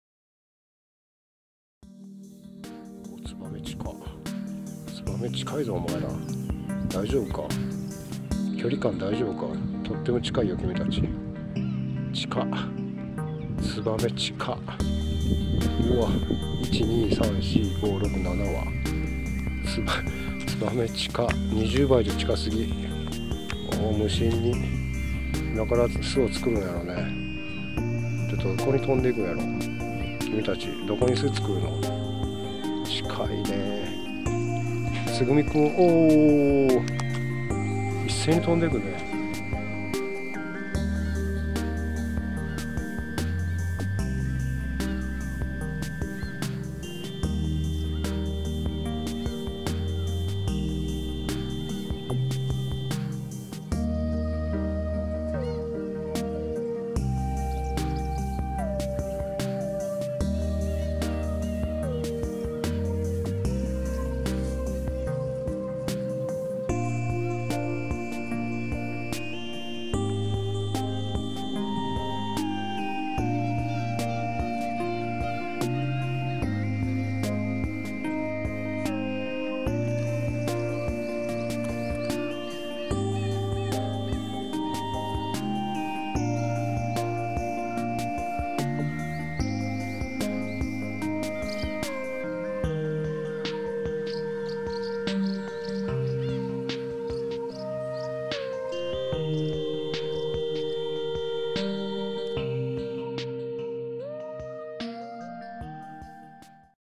春の風物詩 つばめたちの近すぎる人間との距離感！
そろそろ渡りをするツグミたちが集合している姿を撮っていると、自分のすぐ近くにスズメたちが集合していた。明らかに巣の材料を口にくわえている様子。これから巣作りだな(^_-)